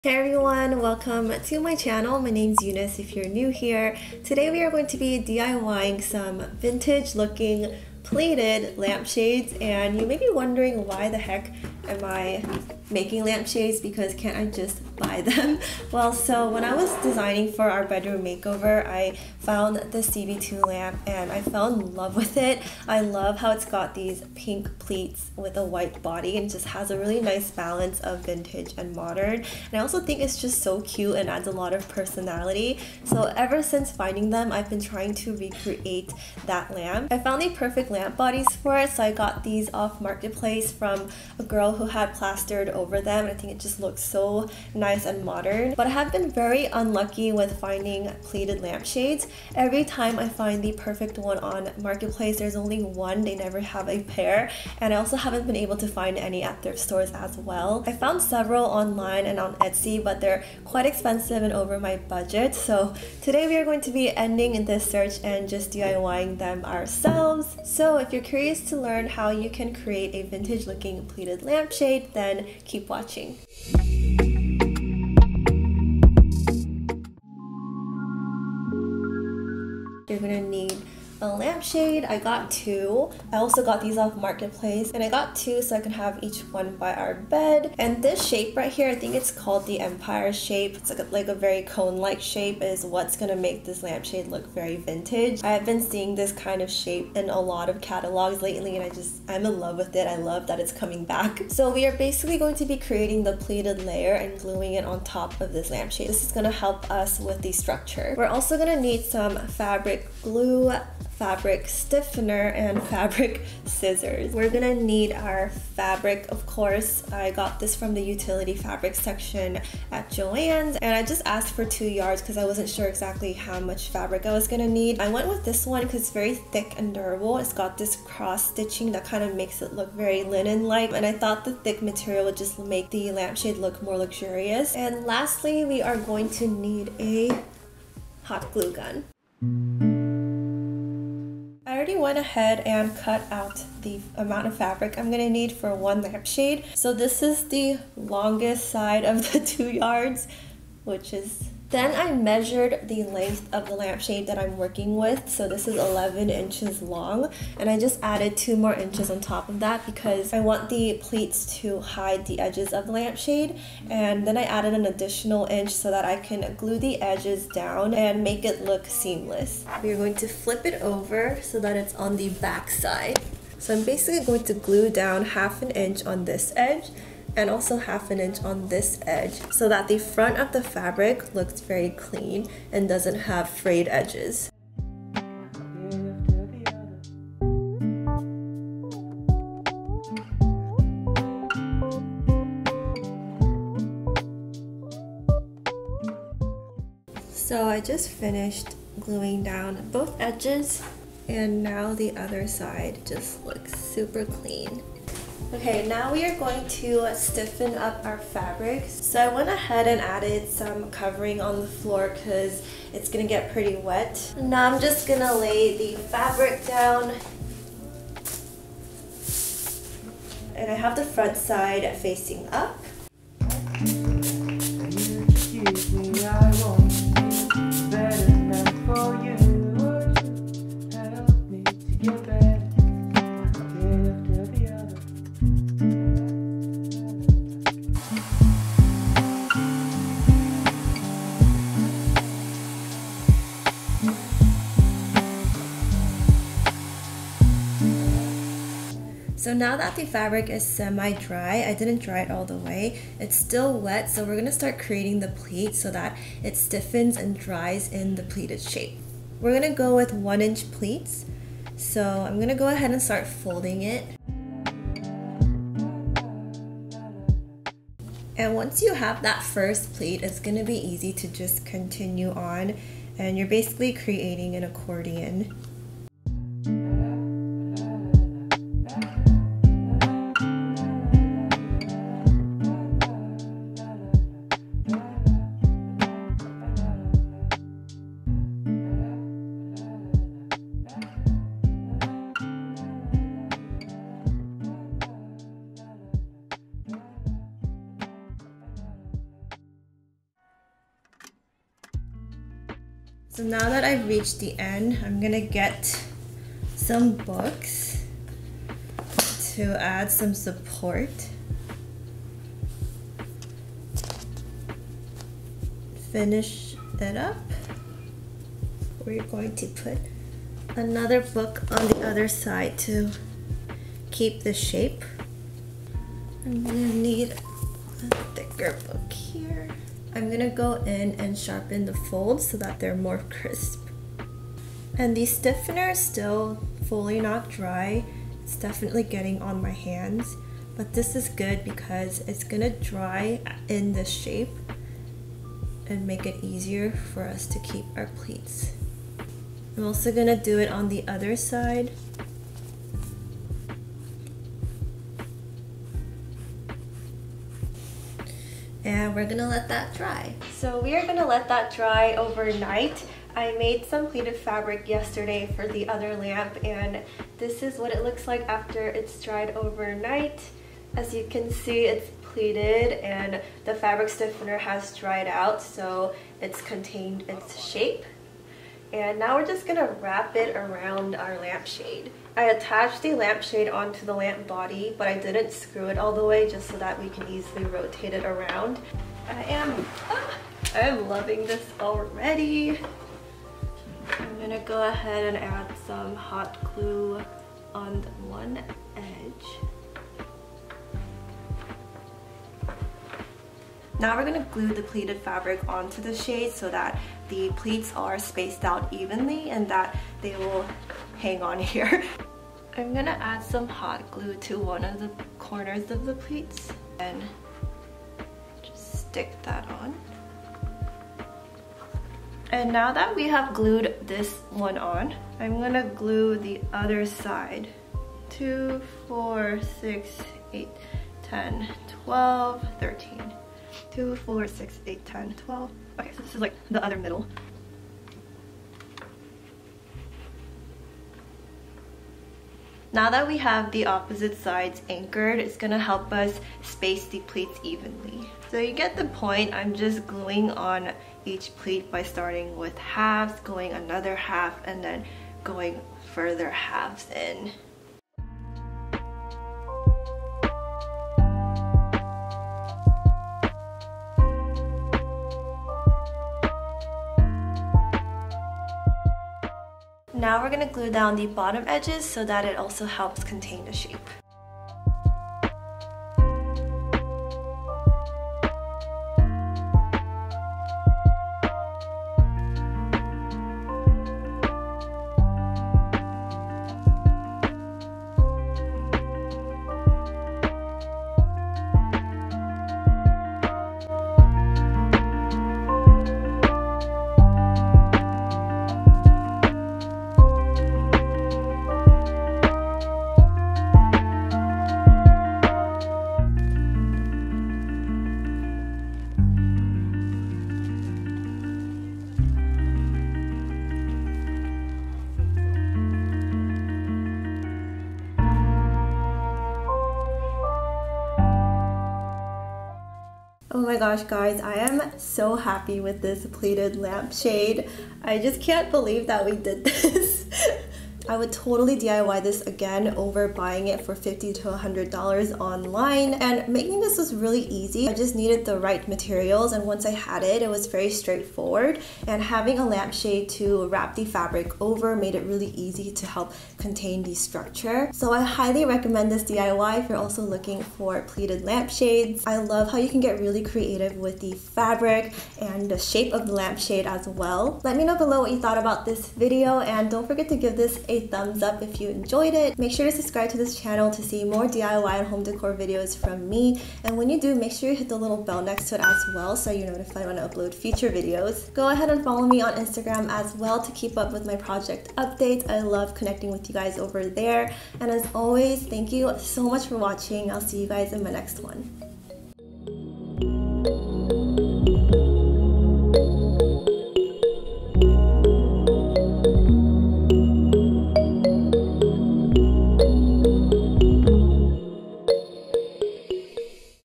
EASY, No Sew DIY PLEATED lamp shade!
I've been searching vintage looking pleated lamp shades for so long, it was time I just DIY'd it!

Timeline:
0:00 - Intro
1:16 - What you'll need
2:19 - DIY
8:43 - Outro

Project supplies:
⌲ Fabric scissors
⌲ Fabric glue
⌲ Fabric stiffener
⌲ Hot glue gun

⌲ $498 (I know!!) Serena & Lily

Equipment
⌲ Sony RX100M3
⌲ Studio Lighting
⌲ Canon EOS Rebel T6